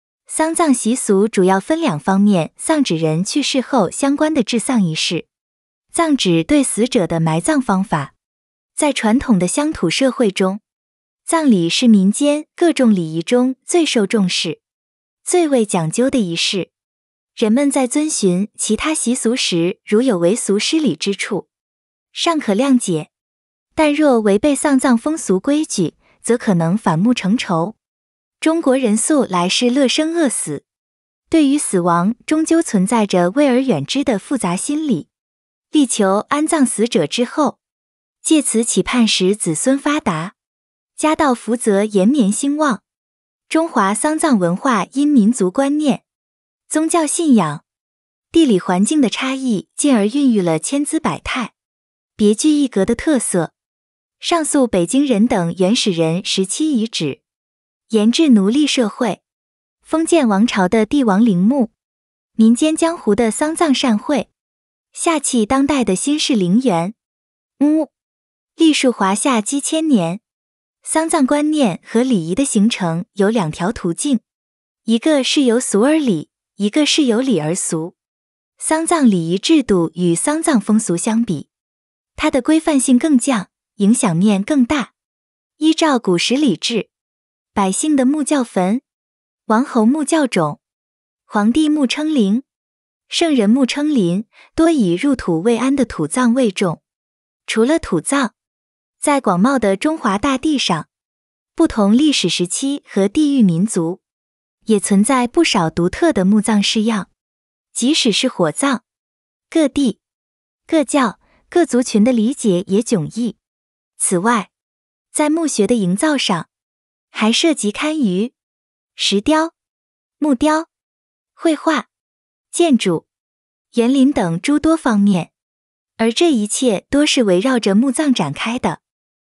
中華喪葬文化的兩大方向
喪葬習俗主要分兩方面──「喪」指人去世後相關的治喪儀式，「葬」指對死者的埋葬方法。在傳統的鄉土社會中，葬禮是民間各種禮儀中最受重視、最為講究的儀式，人們在遵循其他習俗時如有違俗失禮之處，尚可諒解。但若違背喪葬風俗規矩，則可能反目成仇。